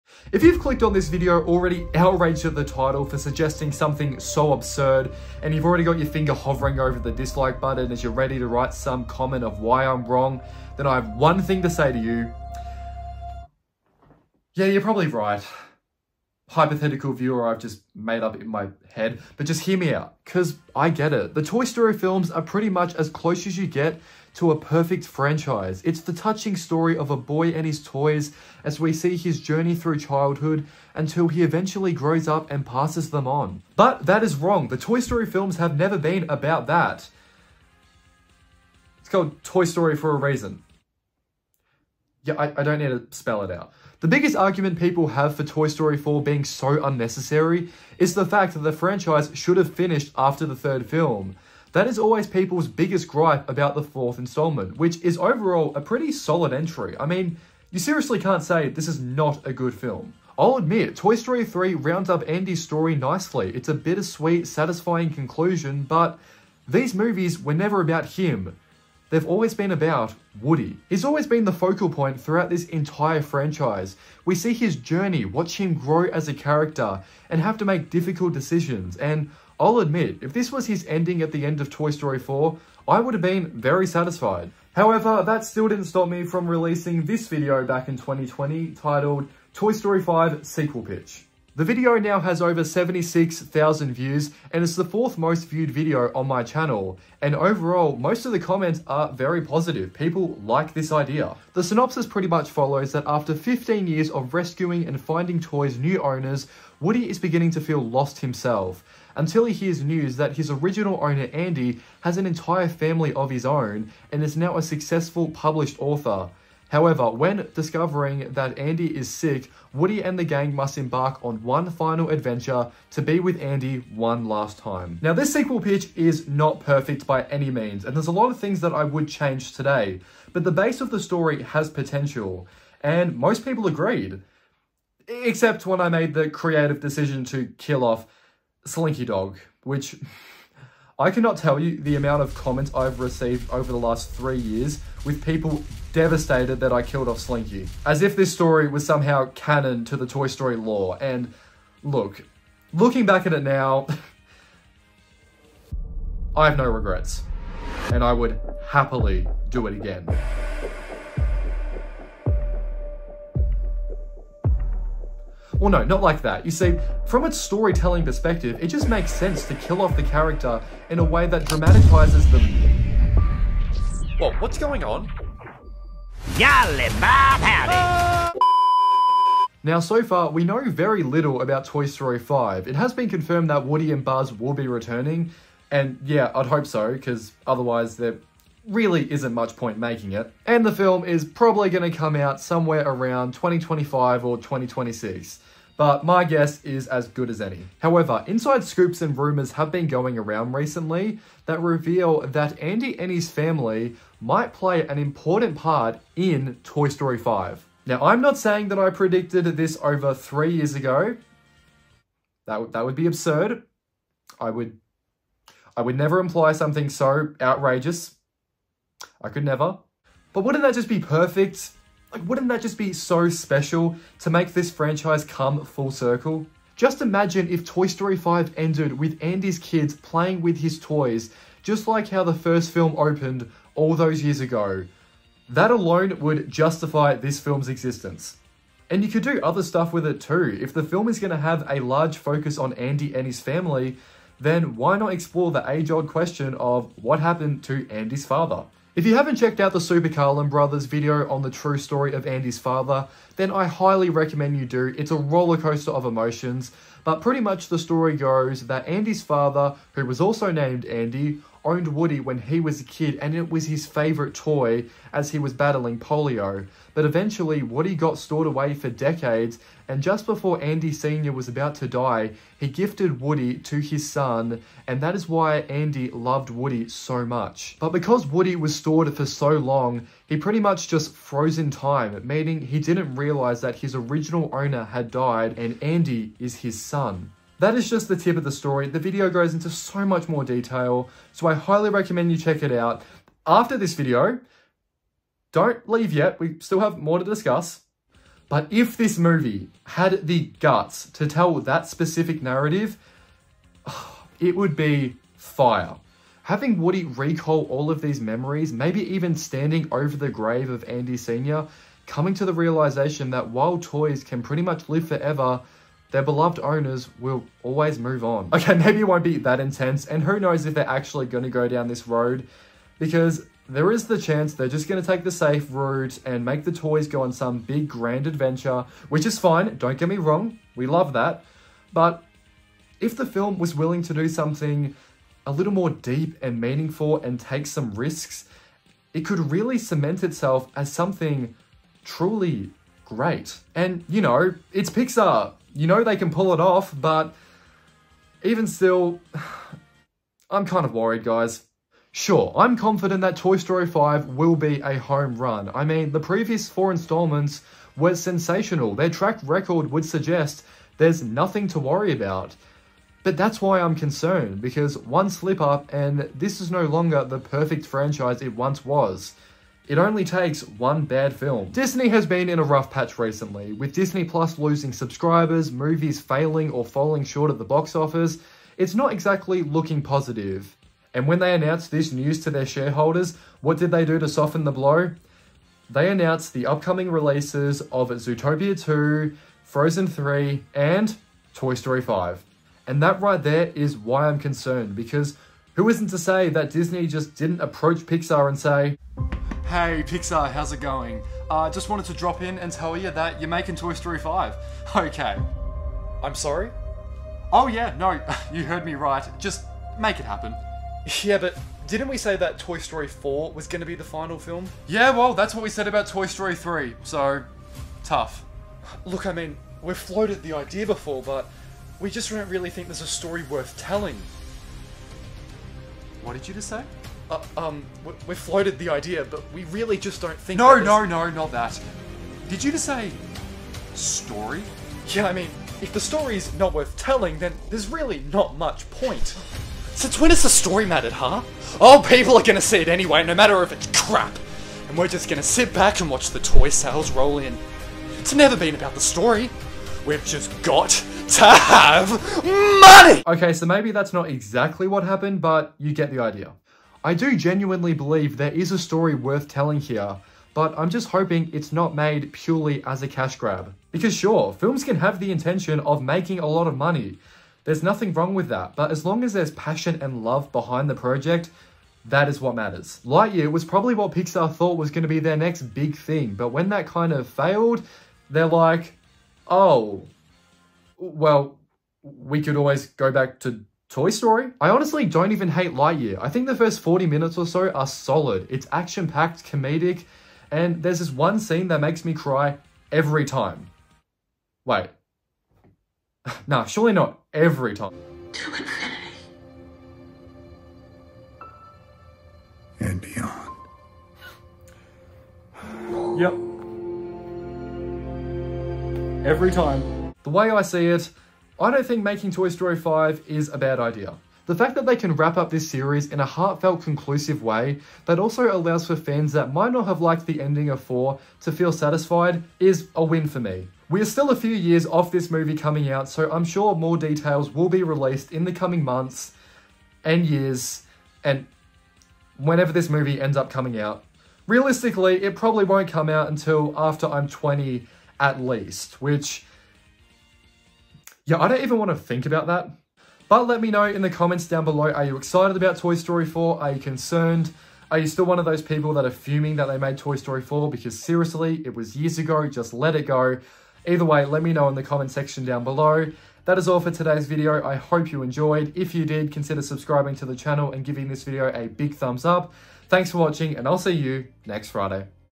Toy Story 5 Needs to Happen…
With the recent announcement of Toy Story 5 it has many fans questioning what is even the point of continuing this franchise again. However I believe that Toy Story 5 won’t just wrap up this series, but could be it’s most vital and important film yet.